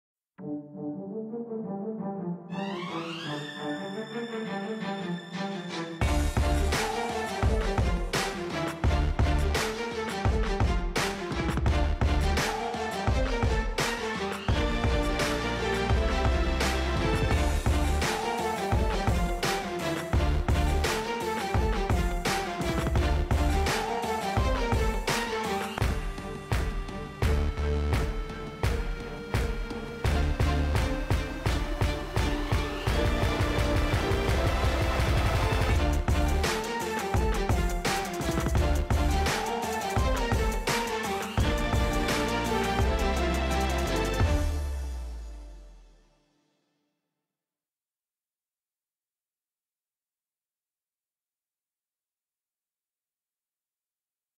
The Remix: Joint Practice with Texans
Check out top highlights as the 49ers and Texans hit the field for the first-ever joint practice at the SAP Performance Facility.